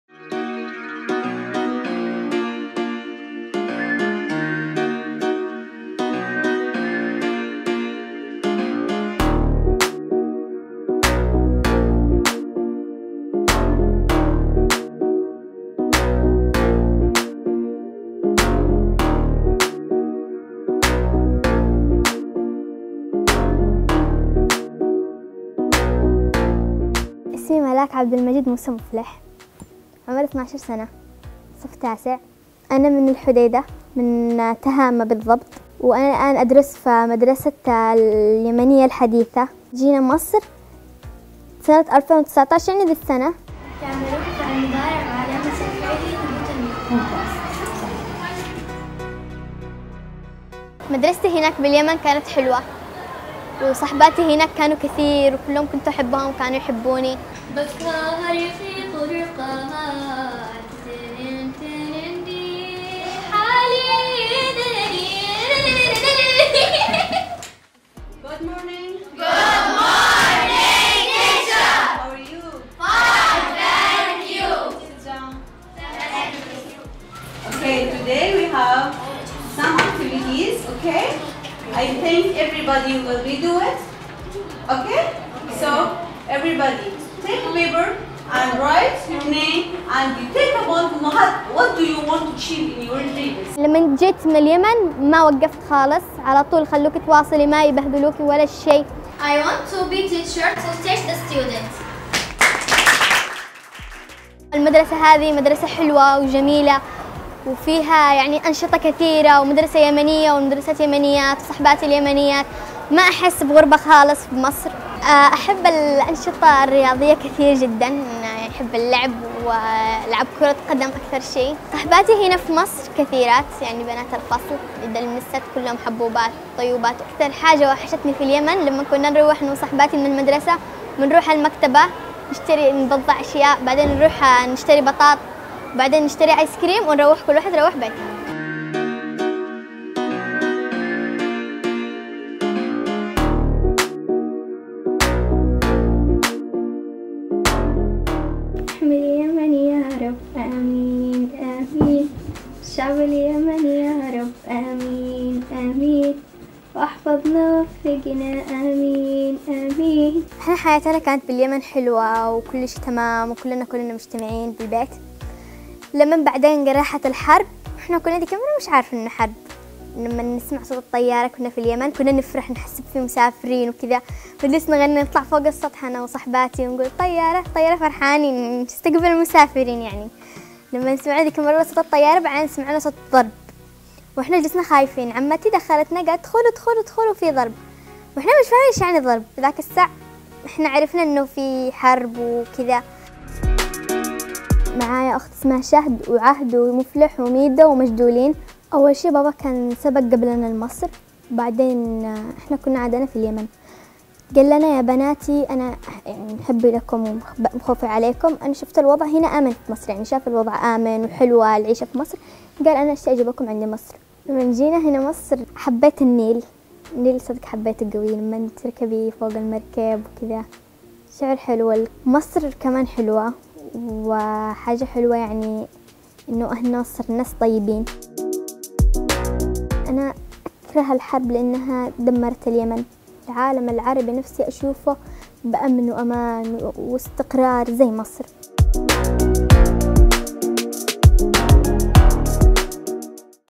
"ملاك اليمن" فيلم لـ اليوم السابع يعكس اندماج اليمنيين بمصر
يسلط اليوم السابع الضوء على قصة ملاك اليمن كنموذج للطلاب اليمنيين فى مصر والذين انتقلوا إليها ليحيوا بشكل جديد بعيدا عن الحرب والدمار.